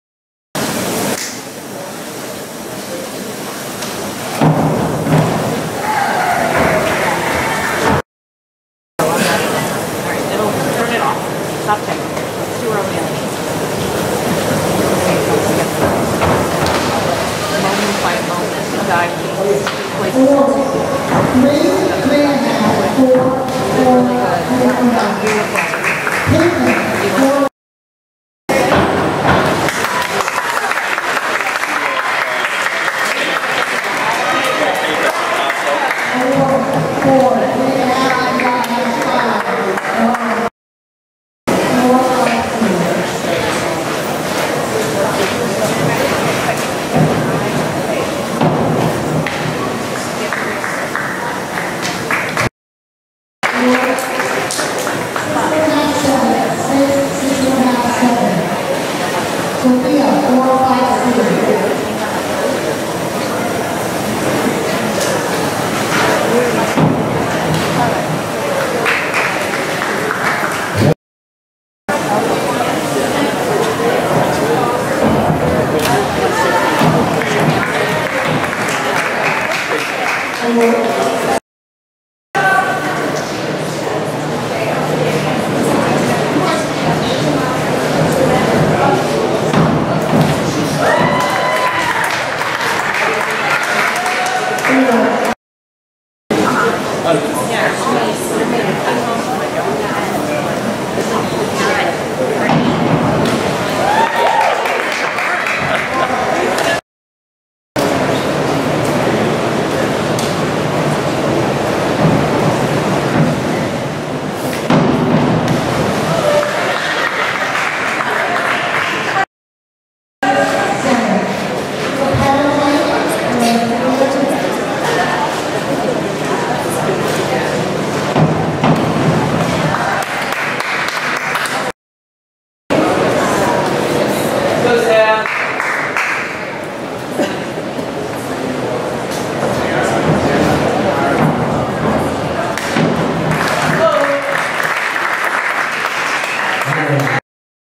Sam Duncan 2017 USA Diving Jr Region 1 Championships 1 meter
Sam Duncan's performance during the 2017 USA Diving Jr Region 1 Championships 1-meter competition, filmed April 23, 2017 in Greenwich, CT. Sam earned the bronze medal, finishing in 3th place out of over 30+ competitors with 463 points/11 dives; the top 15 advance to the Zone 1 competition in Huntersville, NC in June 2017.